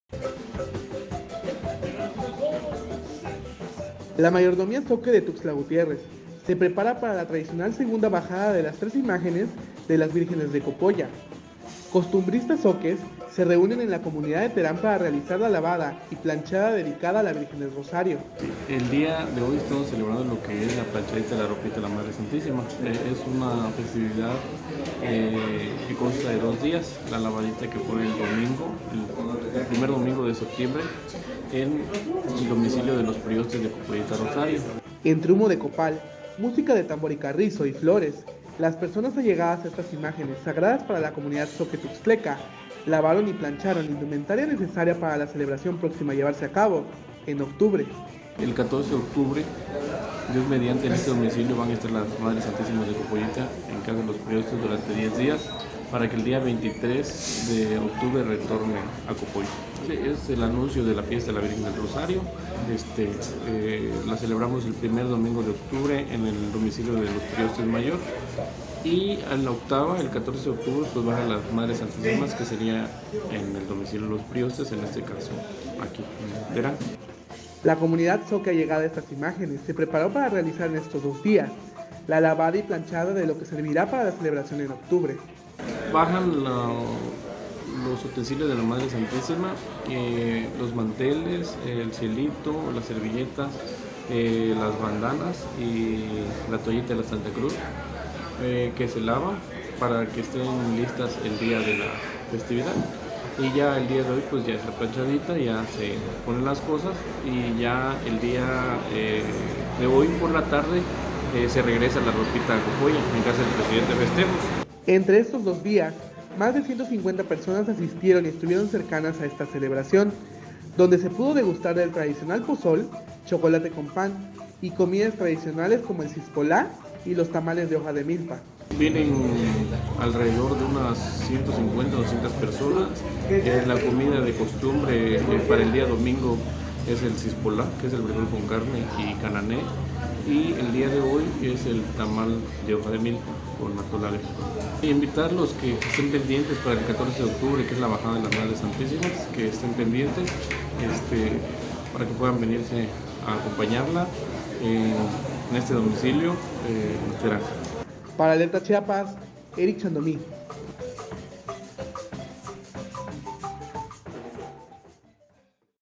Vírgenes de Copoya serán bajadas en octubre
La mayordomía zoque de Tuxtla se prepara para la segunda bajada de las vírgenes. Costumbristas se reúnen en la comunidad de Terán para realizar la lavada y planchada dedicada a la virgen del Rosario.